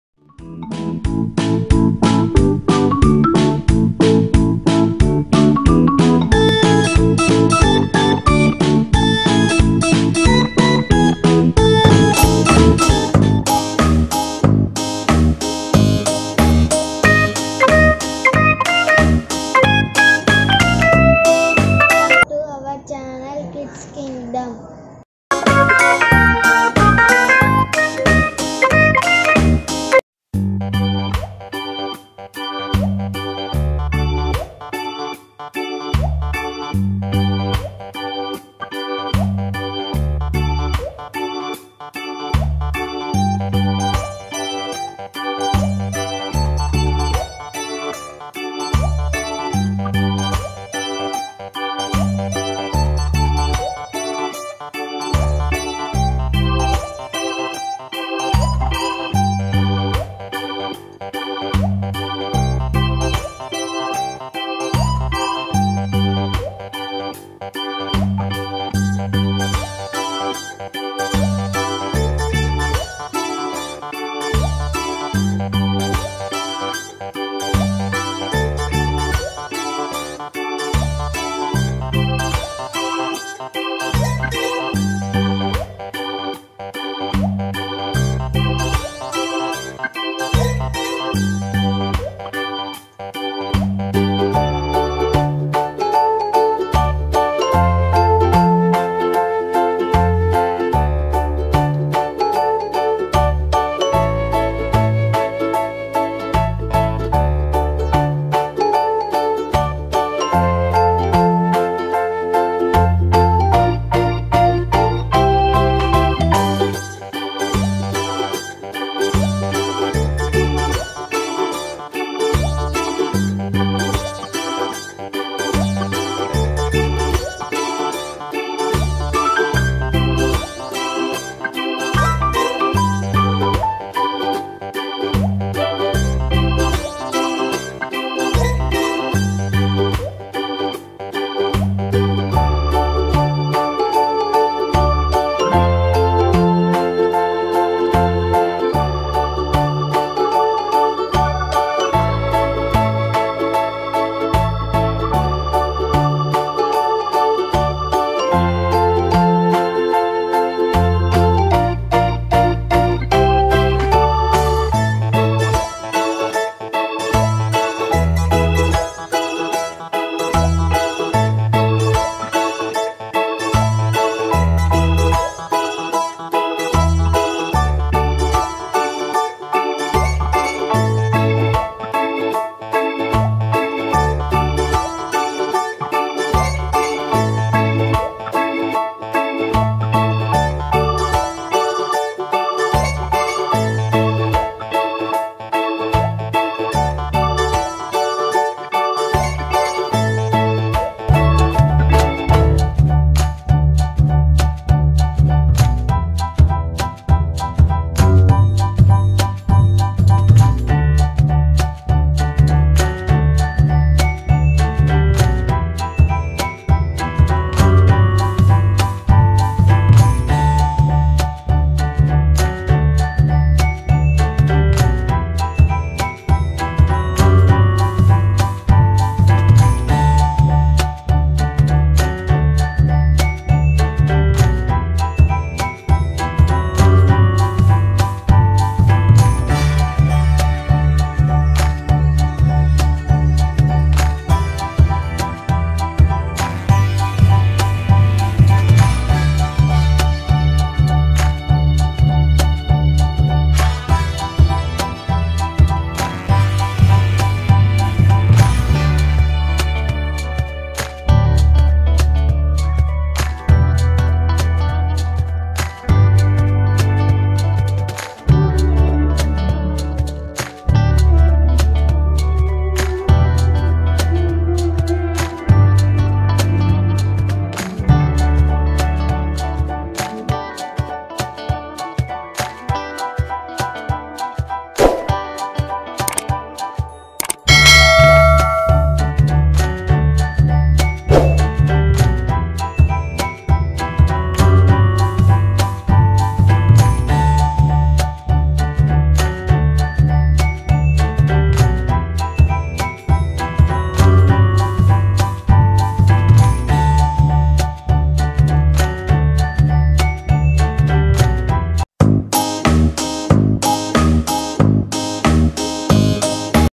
PENCIL SHAVING ART | BEST OUT OF WASTE | PENCIL CRAFT IDEAS FOR KIDS | KIDS KINGDOM
Hi Kids
lets learn pencil craft ideas for kids, best out of waste.

Please follow us in Facebook :-
inappropriate.
you may have rights to the images (or)music, instead of contacting
if you know someone who needs to see it please share it.